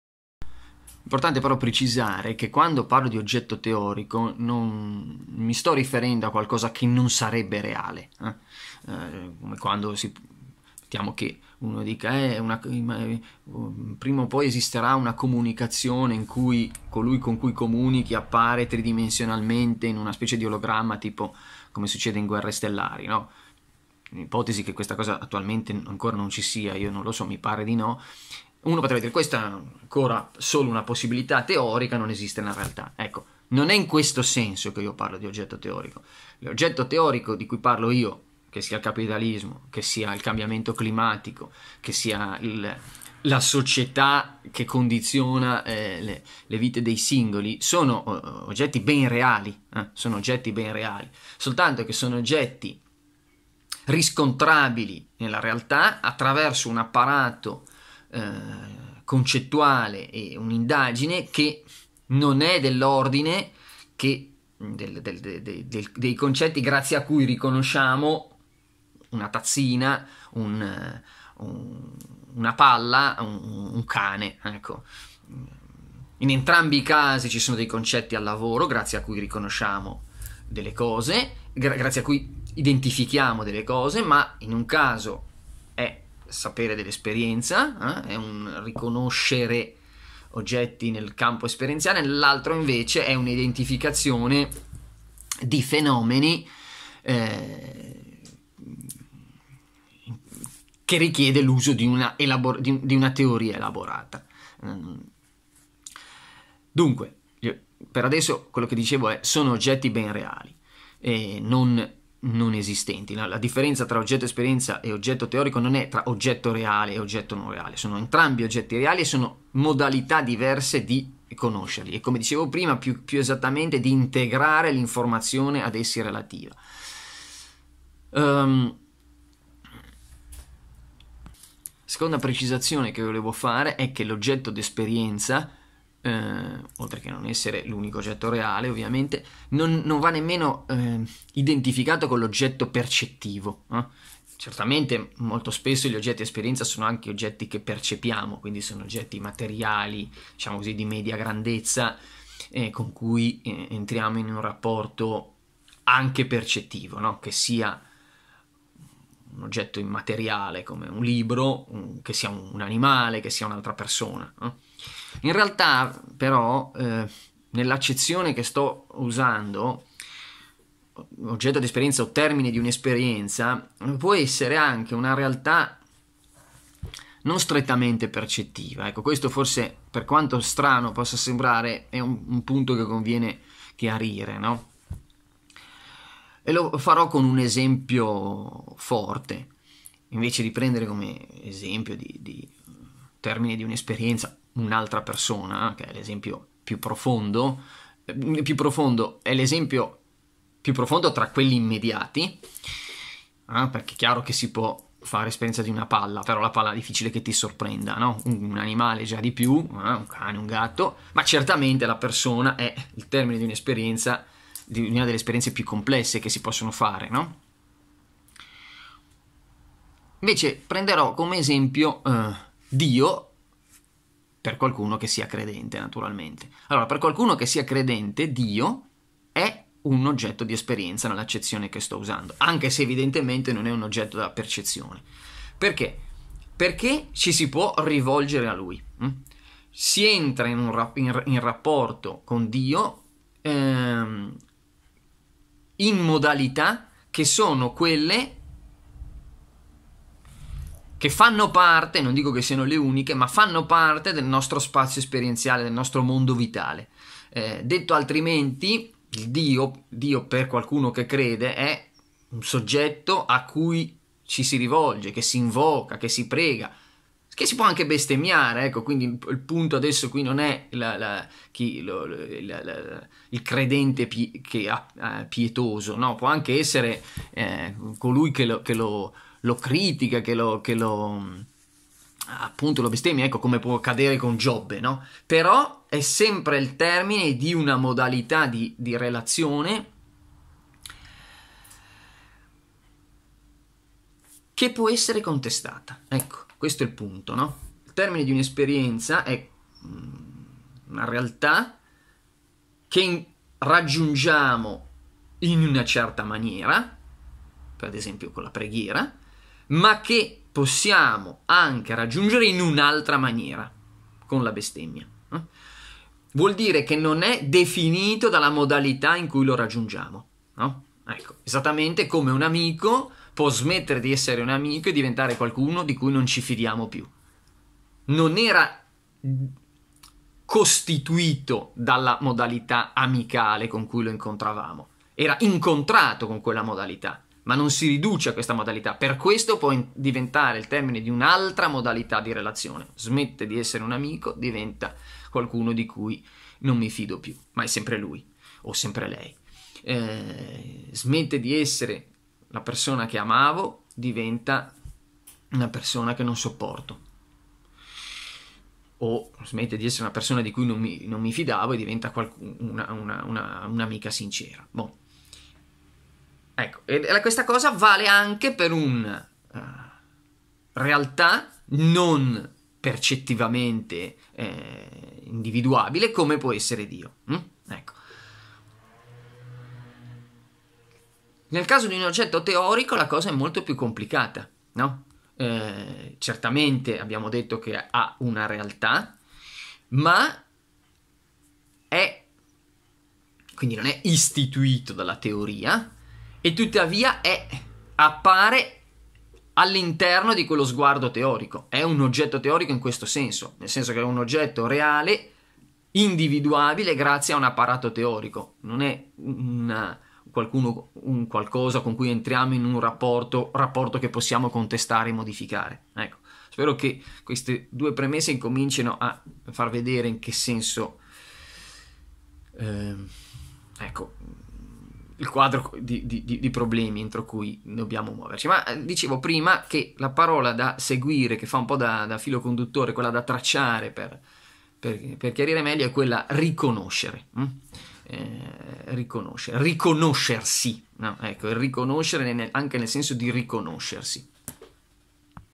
12. Cosa intendo per oggetto di un'esperienza possibile?
Qui introduco due precisazioni, la prima riguarda quello che ho chiamato un oggetto teorico e consiste nel dire che non è un oggetto solo possibile: è un oggetto ben reale, solo che può essere identificato solo grazie a una teoria e dunque applicando i concetti tecnici di cui è intessuta. La seconda riguarda quello che ho chiamato l'oggetto o il termine di un'esperienza possibile: preciso che non è necessariamente un oggetto percepibile, cioè che non è il suo essere percepibile che lo rende esperibile. Piuttosto, il punto è che fa parte del nostro mondo pratico-vitale, per cui lo possiamo raggiungere appoggiandoci alla rete di significati e atteggiamenti grazie a cui ci muoviamo in quel mondo.